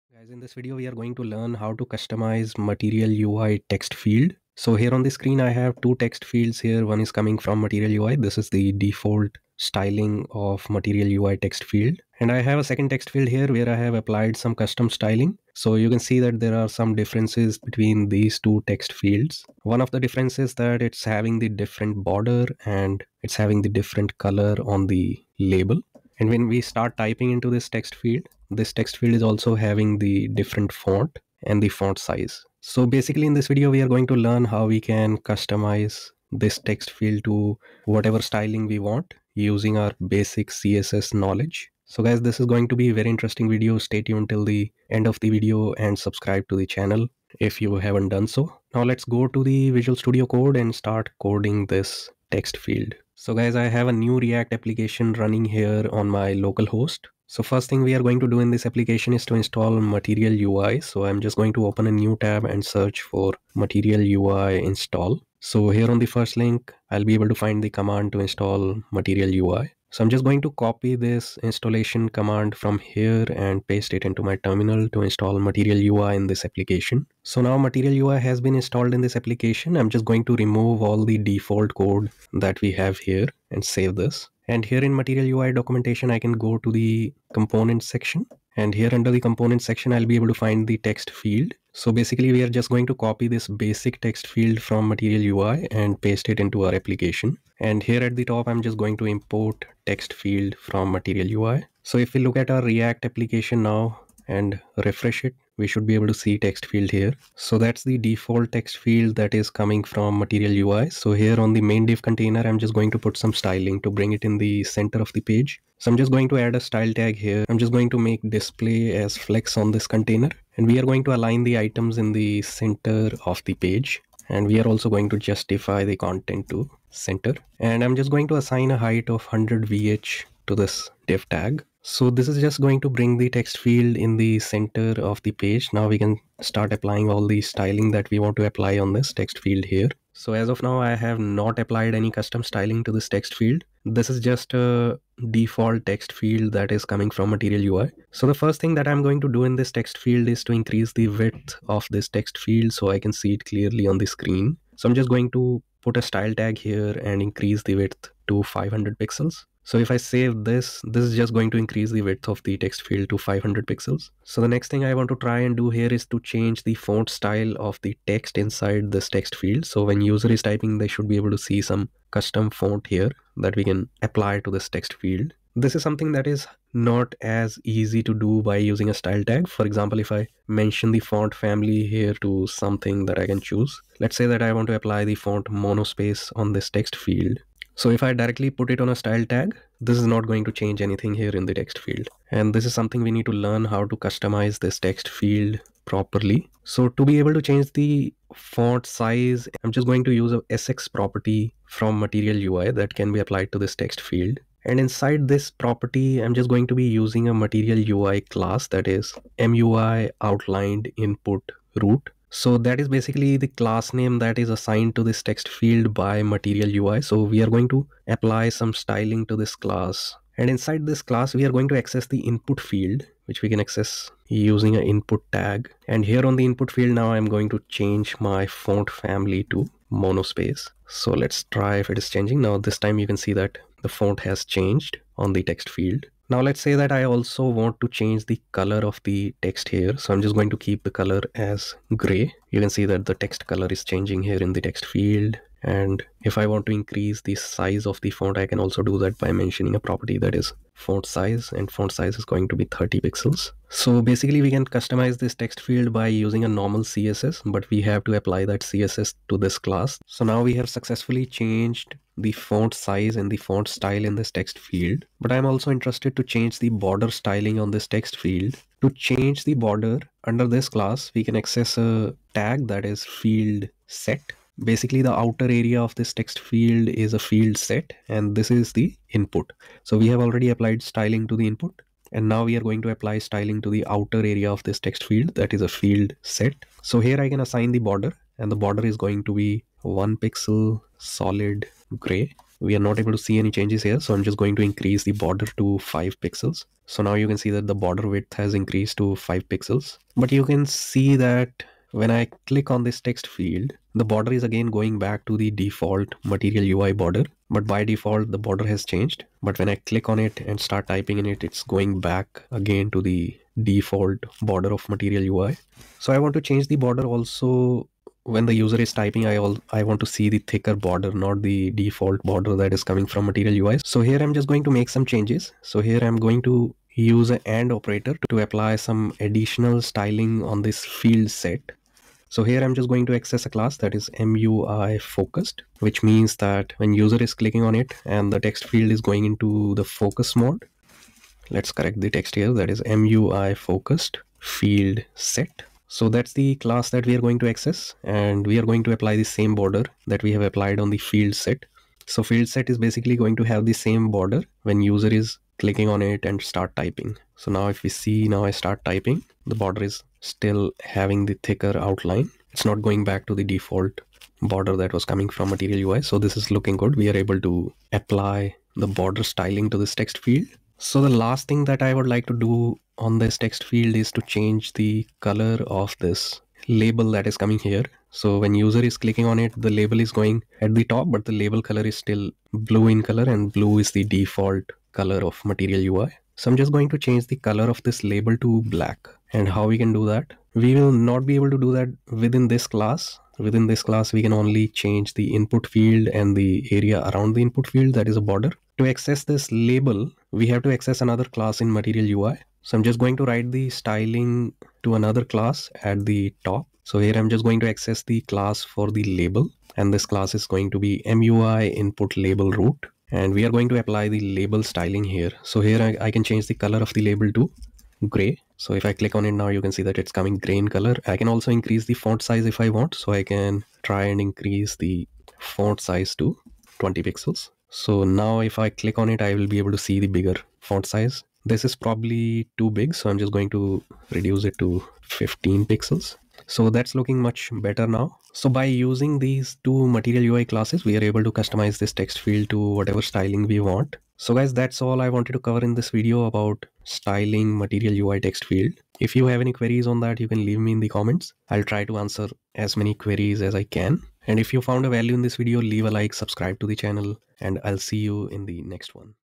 How to Customize Material UI TextField in React JS
In this video, I have explained step by step How to apply custom styling to Material UI TextField.